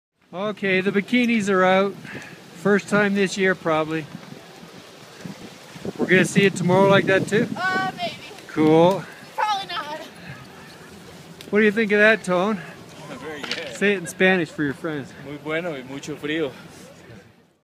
The Bikinis are out on the Slopes at Keystone Ski Resort, Colorado - Mar 27, 2013
The temps warmed up today & brought out the skimpy clothing. Photos of bikinis